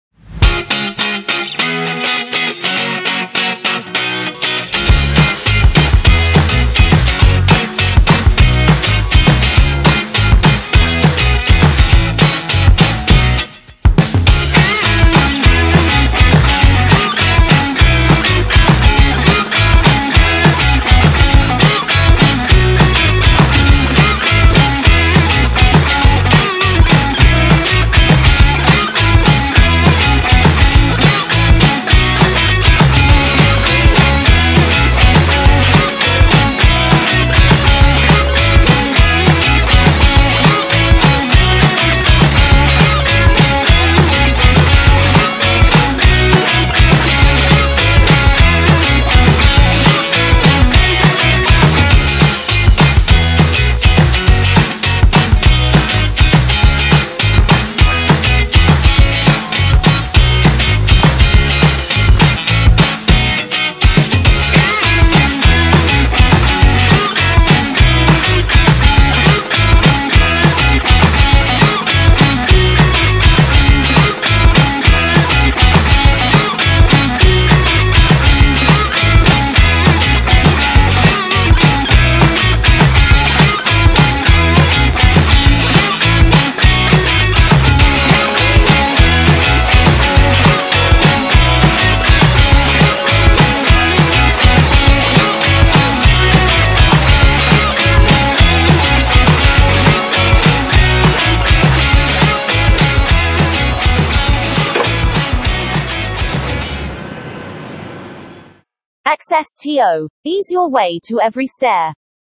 Best Hand Truck For Stairs In 2022
If you are looking for the best hand truck for stairs, you are in the right place.

In this video, we share with you how to move on stairs at home and at work with XSTO stair climbing hand truck.

And you will never worry about your moving work in 2022 with a powered stair climber.